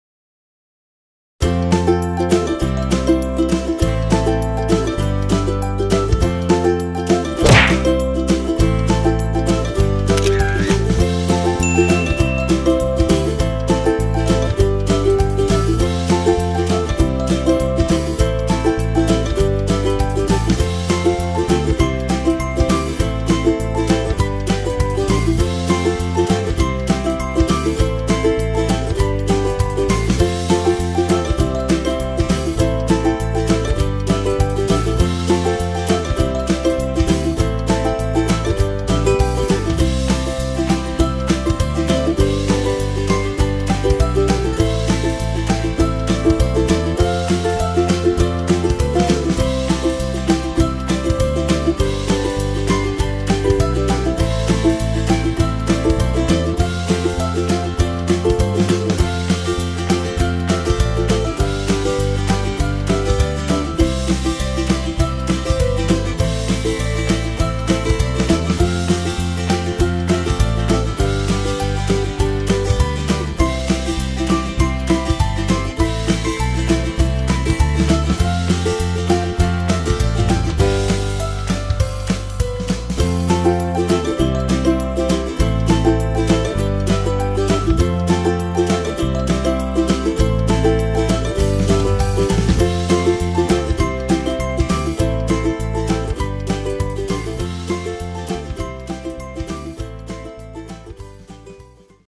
Фото Будка на выставку. Royal Canin выставка Одесса, Николаев.
Фото Будка на выставку. Royal Canin выставка Одесса

ФотоБудка - моментальные фотографии на вашем мероприятии
Пример - Слайд шоу из фотографий с мероприятия.
Фото Будка - это то место на выставке где хочет побывать каждый и получить фотографию на память, ведь все владельцы кошек хотят сфоткаться вместе со своим питомцем на память)
Выставка кошек проходила в Одессе и в Николаеве.